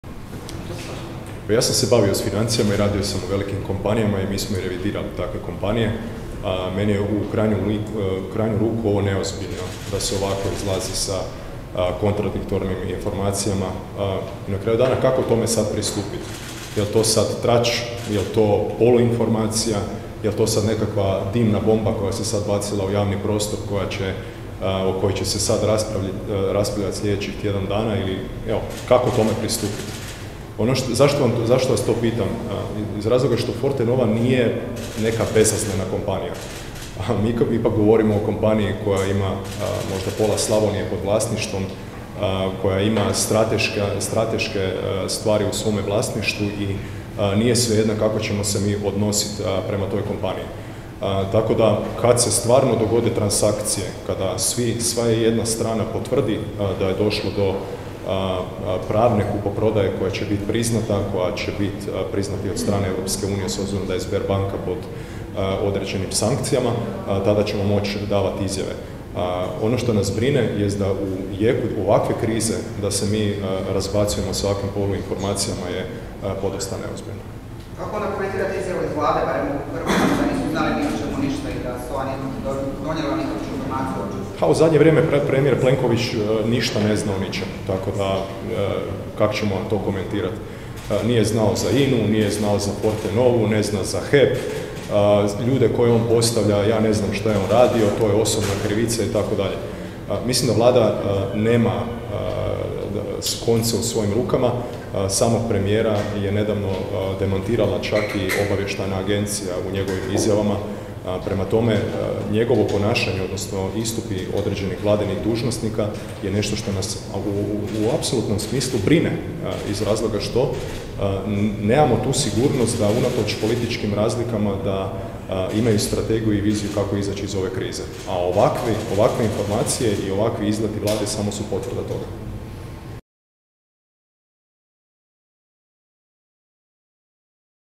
Troskot o Fortenovi: "U zadnje vrijeme Plenković ništa ne zna"
Na konferenciji za medije  održanoj u Saboru slučaj Fortenova komentirao je zastupnik  Zvonimir Troskot.